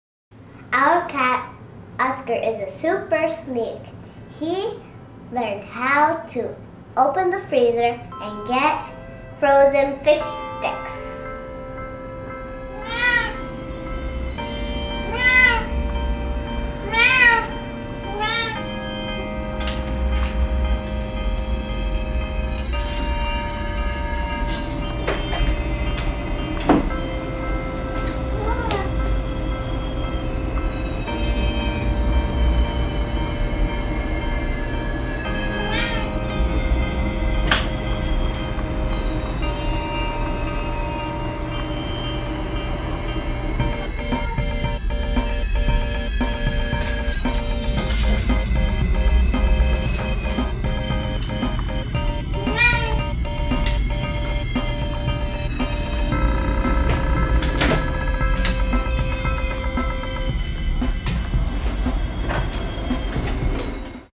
Cat opens the fridge
Don't keep your cat hungry it will teach to open fridge itself